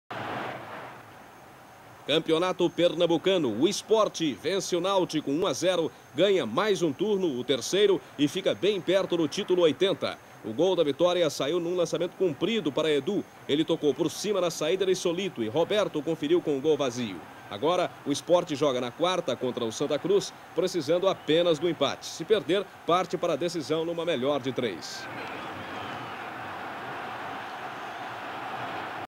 Náutico 0 x 1 Sport | Campeonato Pernambucano 1980
O gol foi marcado na prorrogação. Data da partida: 09/11/1980 | Créditos: Rede Globo